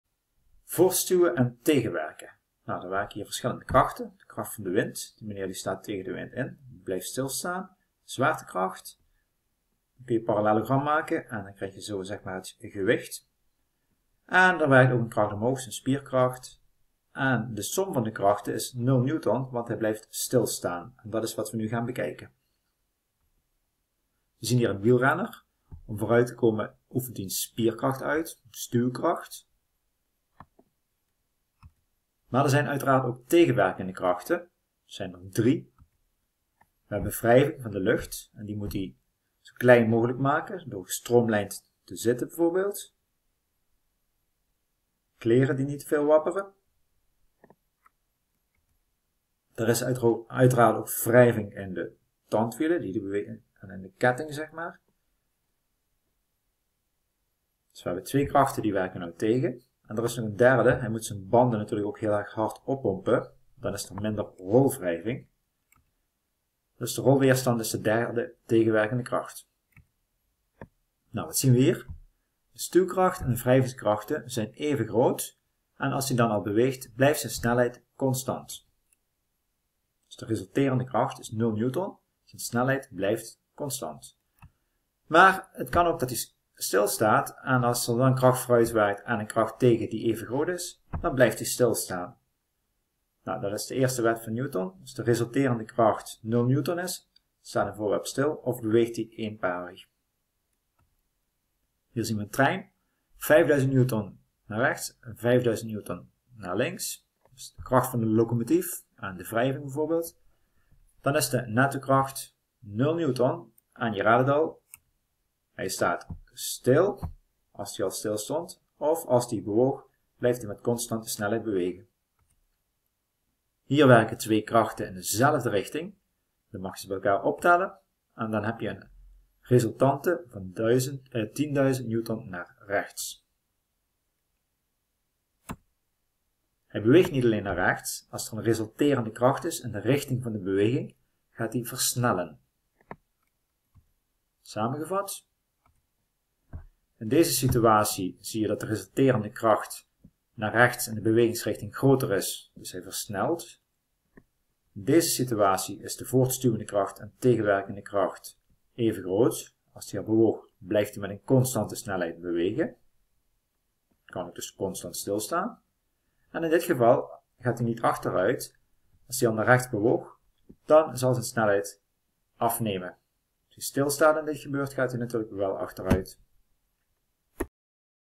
Voortstuwen en tegenwerken deel 2
Uitleg over wat er met de snelheid van een voorwerp gebeurt als de resulterende kracht 0 N is of niet.